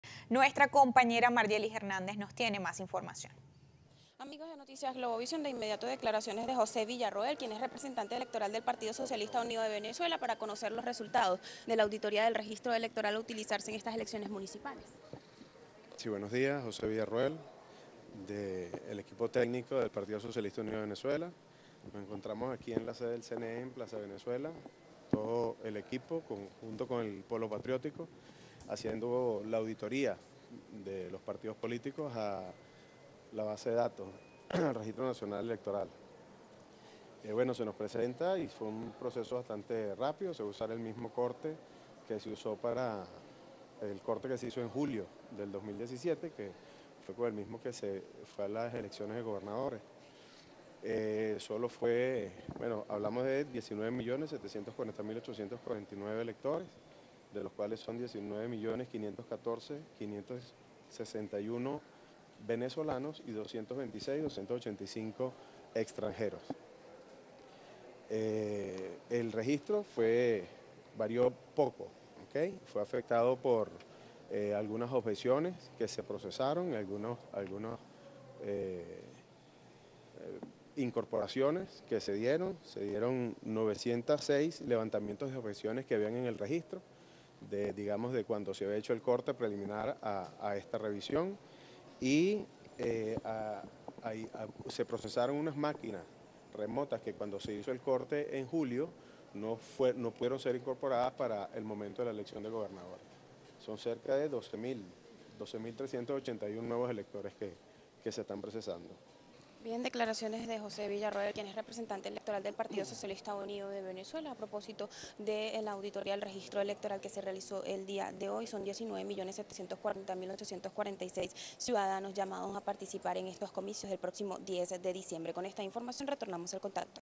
Representante Electoral del Psuv: Proceso de auditoría fue bastante rápido
La tarde de este lunes, José Villarroel, quien es el representante electoral de Partido Socialista Unido de Venezuela, indicó que su "equipo estaba en el Consejo Nacional Electoral (CNE) haciendo la auditoría a los partidos políticos y a la base de datos del registro nacional electoral" y constató que el proceso fue bastante rápido.